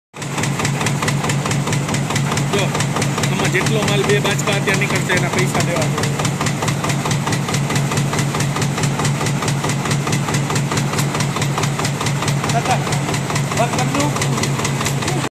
paper cup raw material making machine install in Hyderabad by JP INDUSTRIES call inq +91 9727510387
This machine is high speed metal die cutting machine for paper cup raw materiel